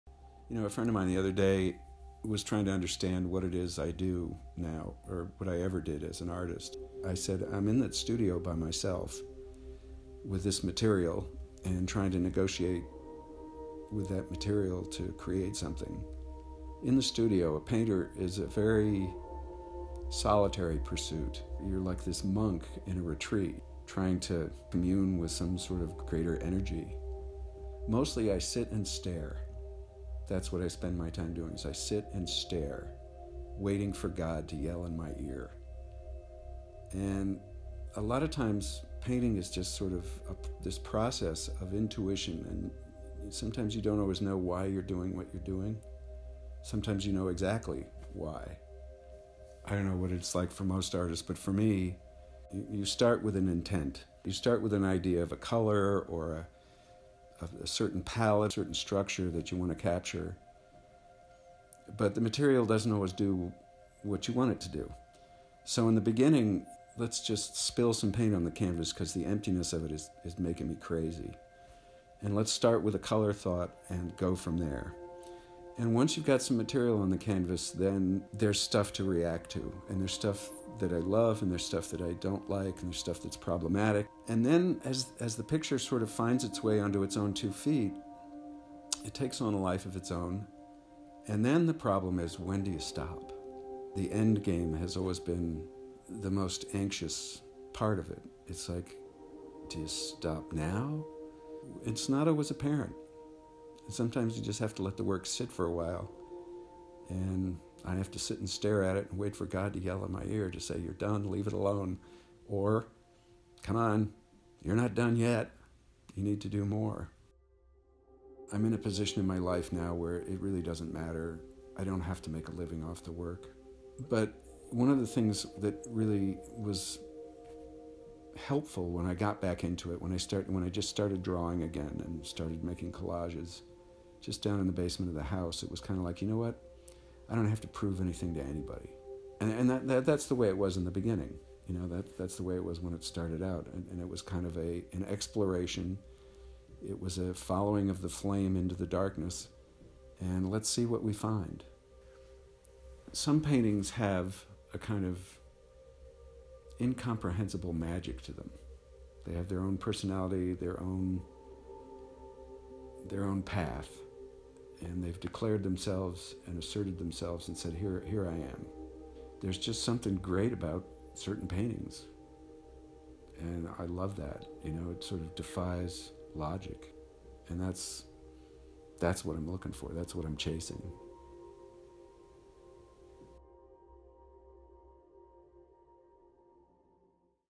Frank Campion
After a 30-year career in the advertising business, Frank Campion returns to the studio resuming the life of a successful starving artist.